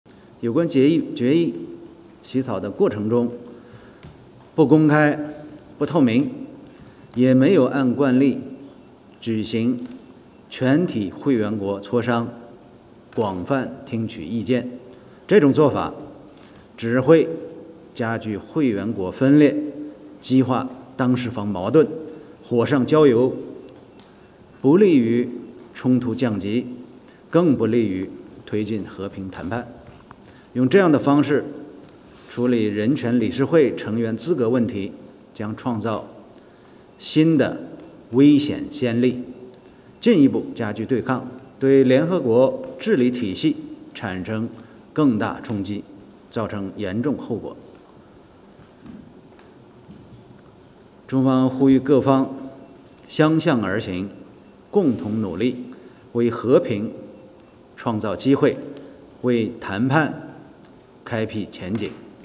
China se opone "a la politización de los DD.HH. y su uso como instrumento de presión"
El Gobierno chino instó a no politizar el ámbito de los derechos humanos y no utilizar esta cooperación como herramienta de lucha, comentó el 8 de abril el portavoz del Ministerio de Asuntos Exteriores chino, Zhao Lijian, en una sesión informativa periódica sobre la resolución que suspendió la pertenencia de Rusia al Consejo de Derechos Humanos de la ONU. Asimismo, Pekín insiste en que el proceso de redacción de las resoluciones correspondientes "no fue abierto ni transparente, ni se celebraron consultas con todos los Estados miembros, como es habitual, ni se escucharon ampliamente las opiniones".